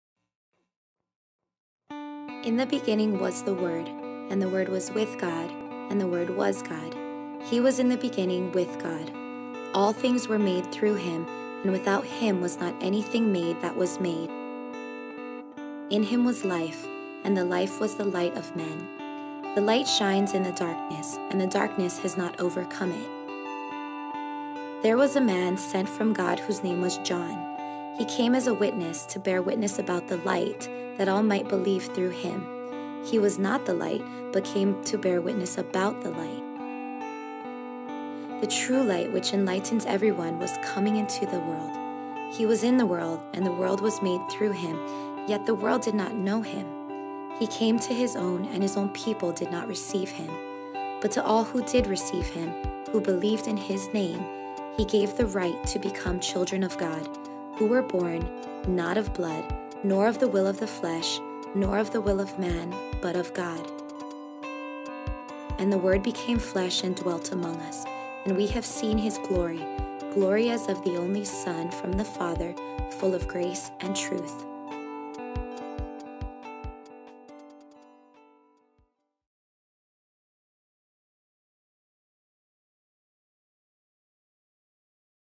John 1:1-14 - The Word Became Flesh
A creative typography rendering of John 1:1-14 accompanied by an audio reading. Excellent option for exposing students to the text. (1:26)